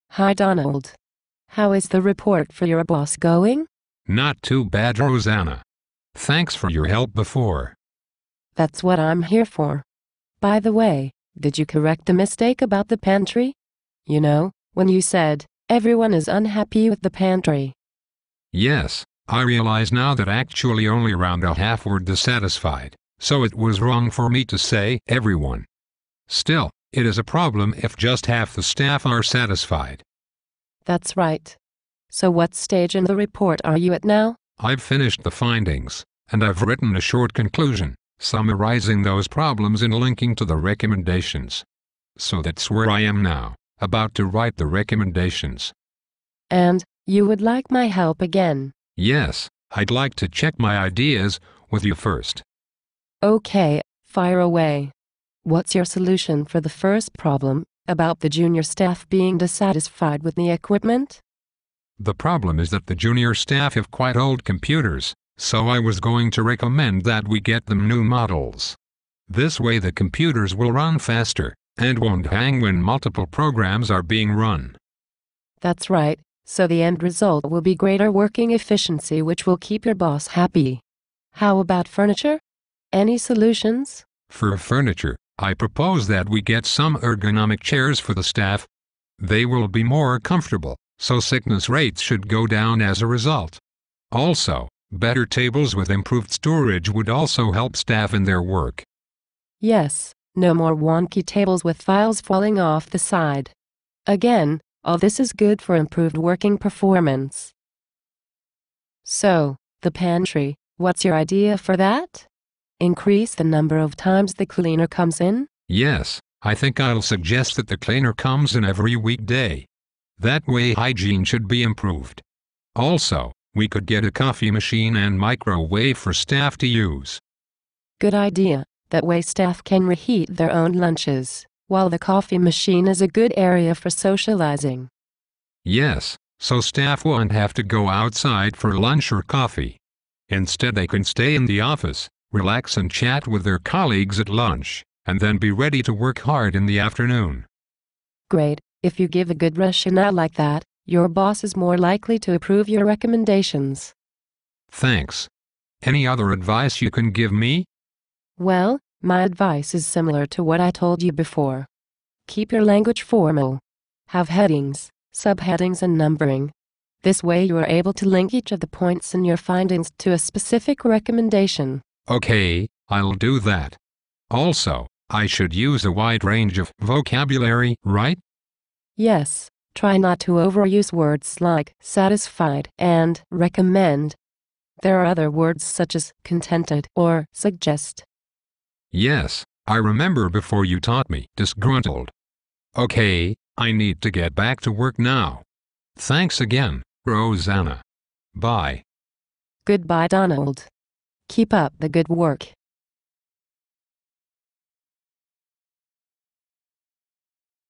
Staff Survey about Working Conditions: Recommendations
Click on captions for English subtitles.

Donald goes back to his mentor Rosanna to further advice about a report he has to write for his boss. this time he asks about the Recommendations.

Look in unit 3 of eLearn for the original table. How would you solve each problem that Donald identified?